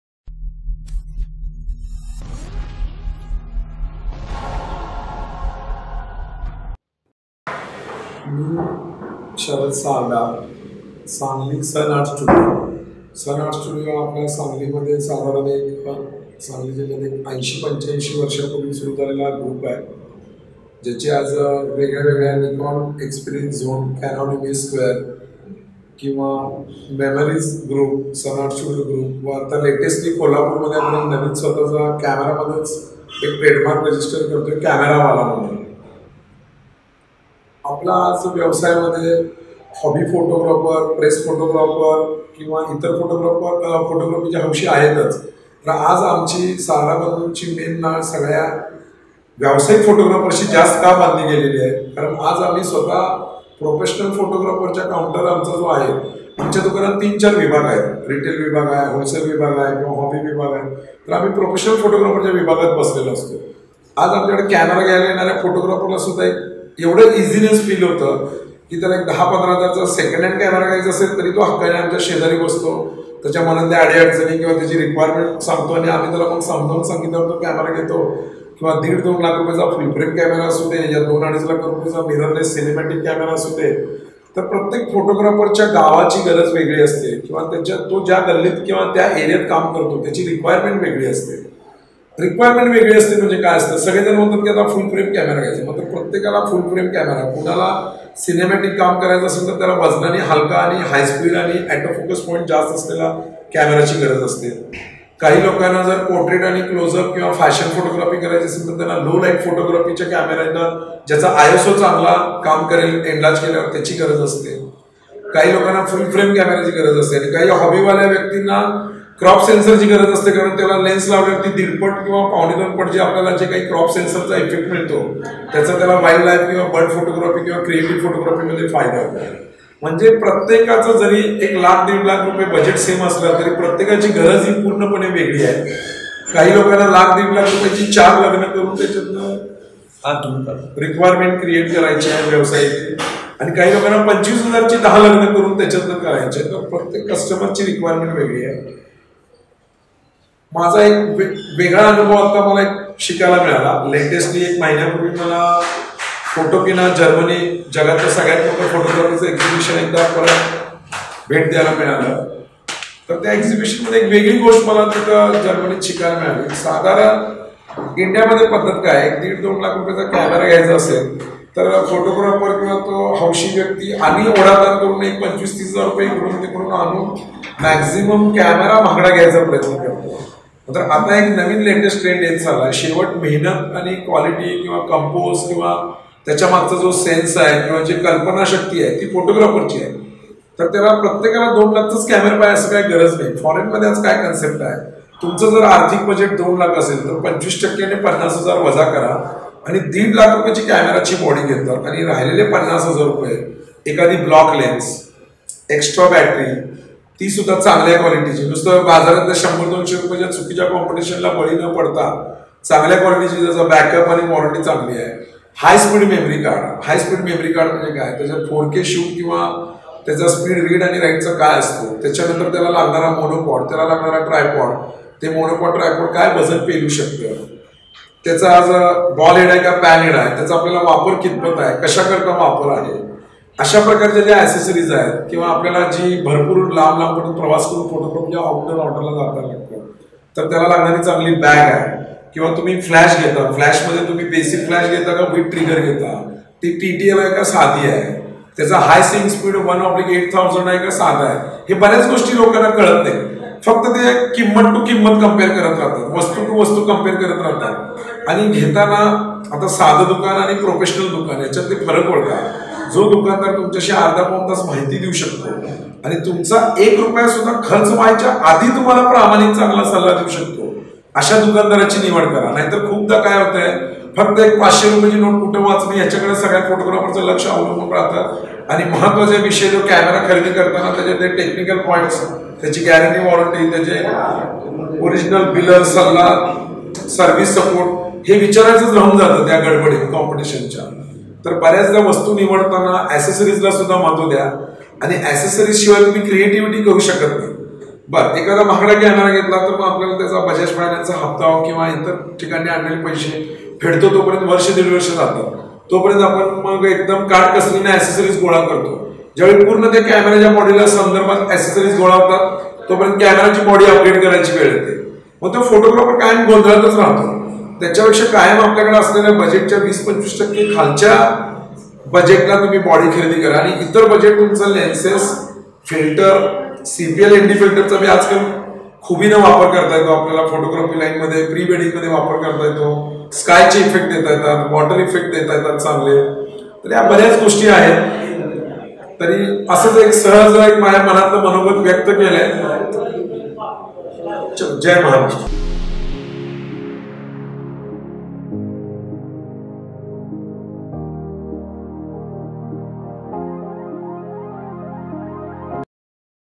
what are the things needed to buy a dslr camera? | Sharad Sarda | Marathi
which camera should i buy?
which camera to buy?
which camera is best?
Mr. Sharad Sarda from Sangli is a person who has commulative 80+ years of experience.
He explains which camera and which accessories to buy from his experience to professional photographers.